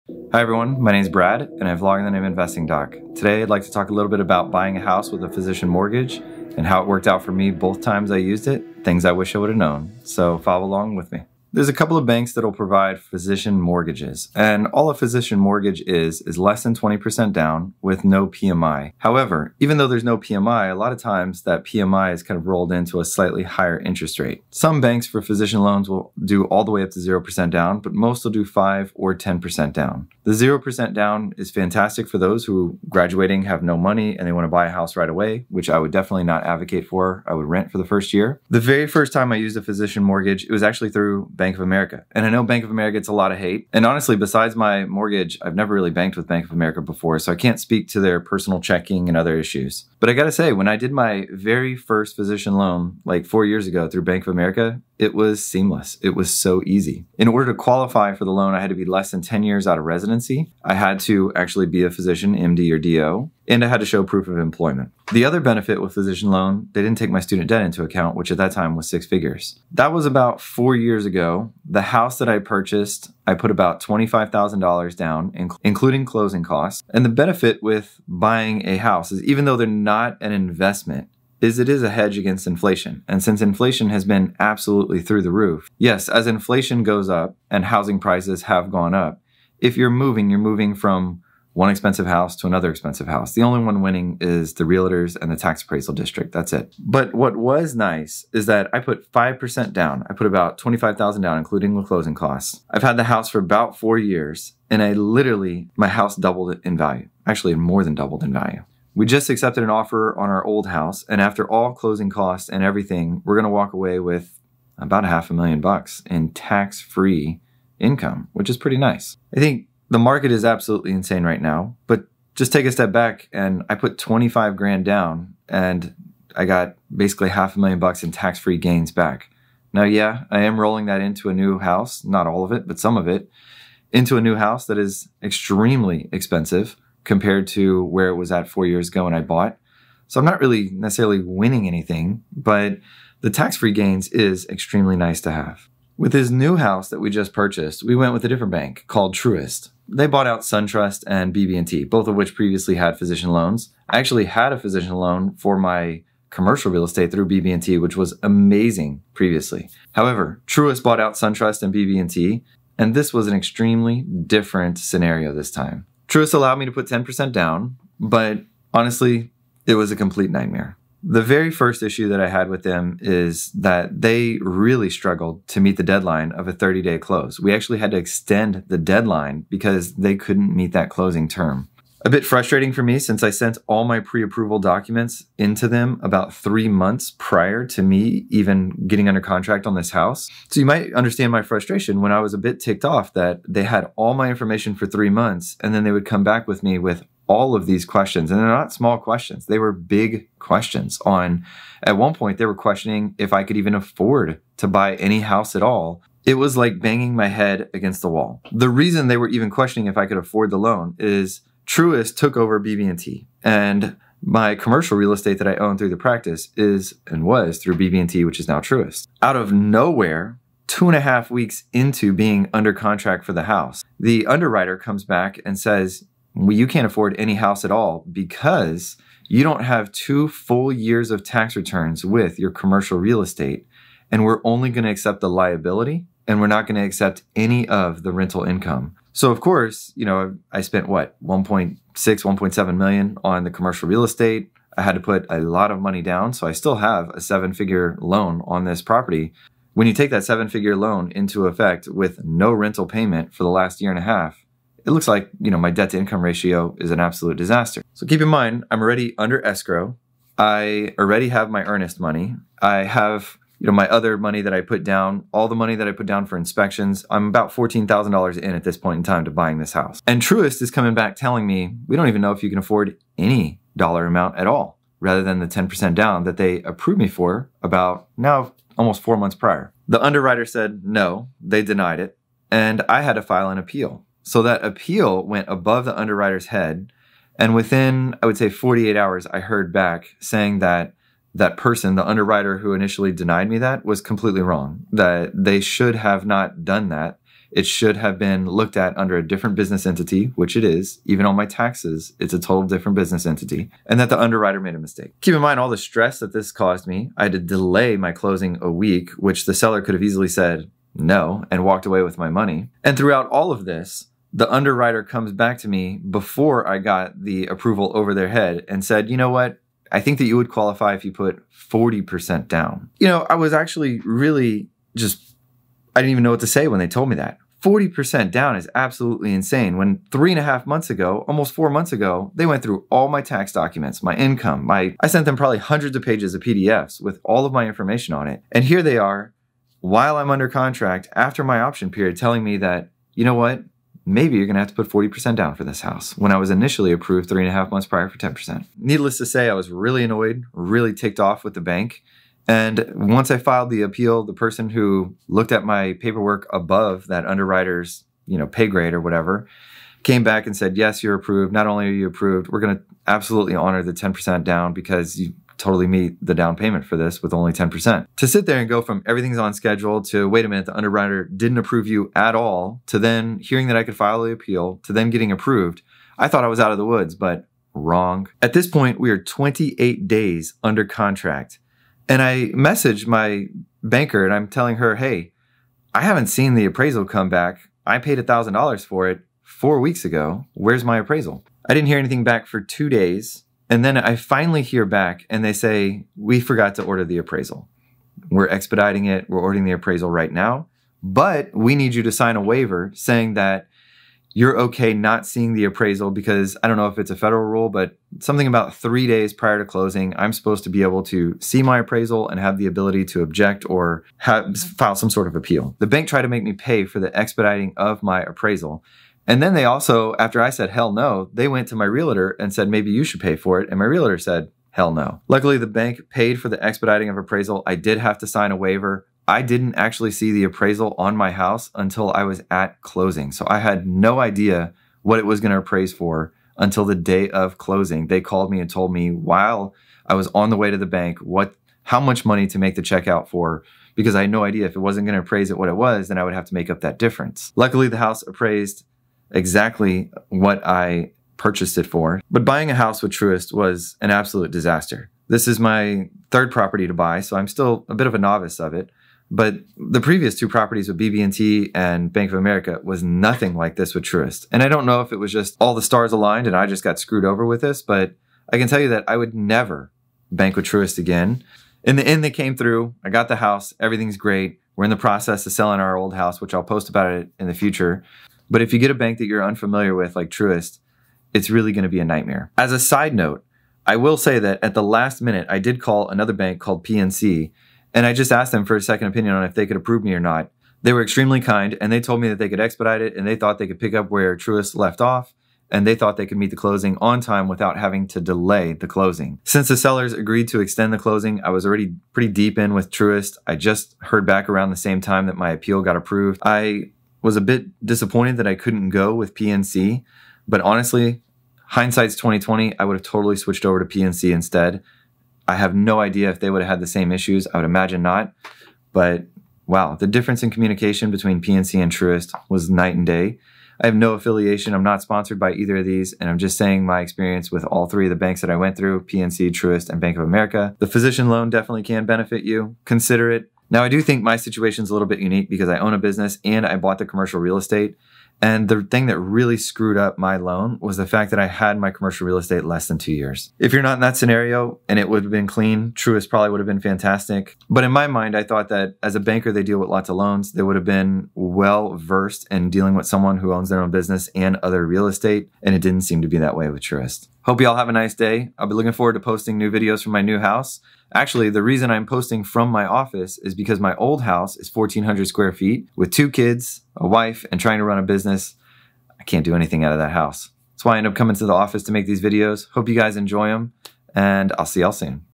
My Doctor Loan Disaster With Truist
Truist bought out BB&T and SunTrust. I decided to use Truist to obtain a doctor loan for my new house. Little did I know that they would have several issues with me purchasing my new house.

We ended up pushing back closing, I had to appeal underwriting decision (and won), and they forgot to order my appraisal.

I will admit that I do think my issues are secondary to me having commercial property. However, being a bank, you would expect them to know how to navigate someone who on paper owns multiple businesses, one being commercial real estate that the other businesses rent from.